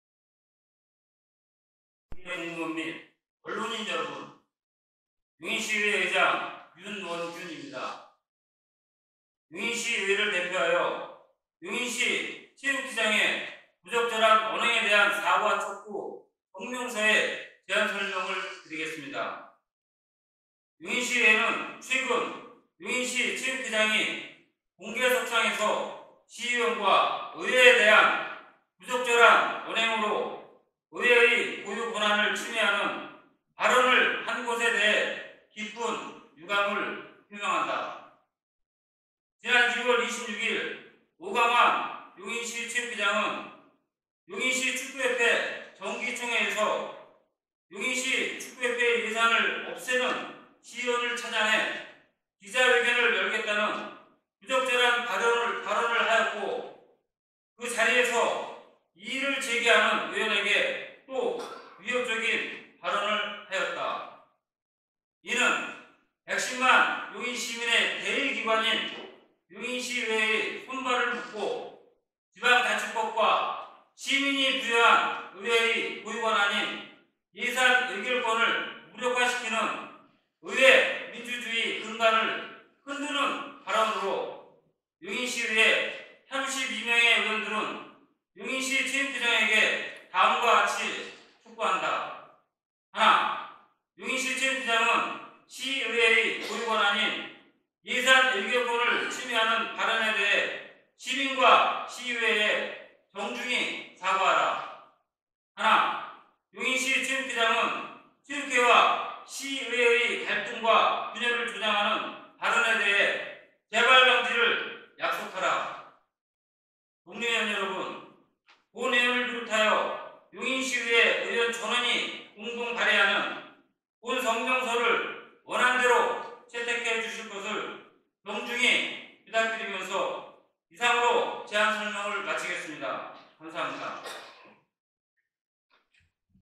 용인특례시의회, '용인시 체육회장의 부적절한 언행에 대한 사과 촉구 성명서' 발표
[양해용 기자]=9일 오전 용인특례시의회 본회의장에서 열린 제271회 임시회 제1차 본회의에서 윤원균  용인특례시의회  의장이  ｢용인시 체육회장의 부적절한 언행에 대한 사과 촉구 성명서｣ 제안설명을 하고 있다.

오광환 용인시 체육회장 "성명서 내용 사실과 다르다. 사실 확인이 필요 하다.